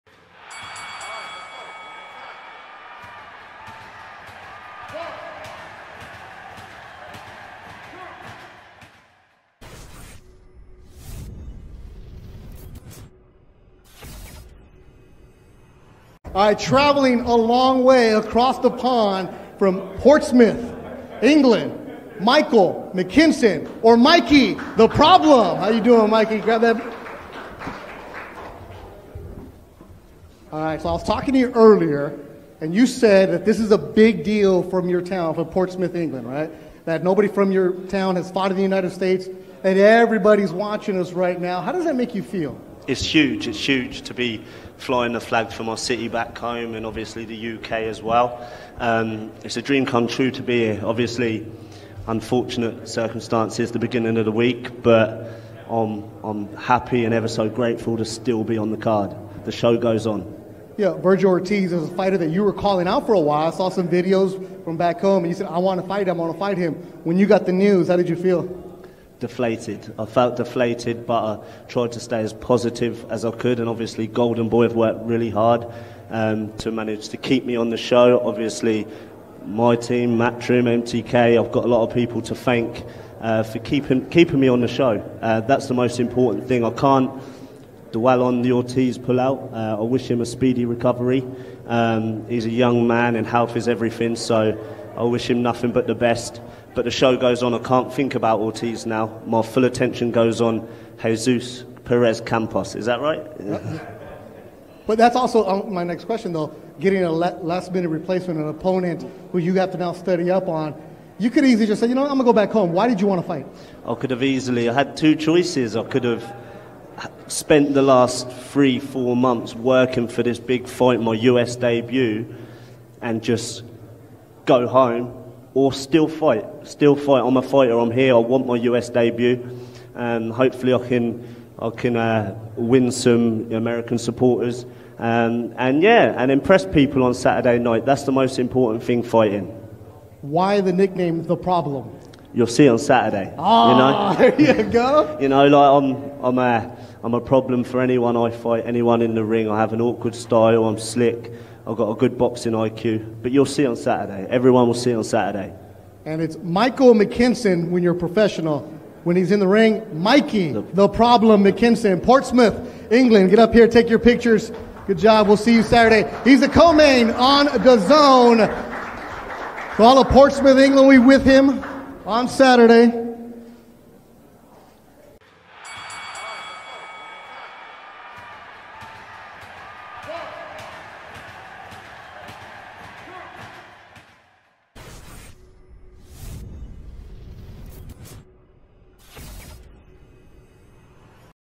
'THE SHOW GOES ON!' - MIKEY MCKINSON TALKS VERGIL ORTIZ PULLOUT & RUSH TO FIND LATE REPLACEMENT
VOTE iFL TV for BEST COMBAT SPORTS PODCAST in the Sports Podcast Awards

FOOTAGE COURTESY OF GOLDEN BOY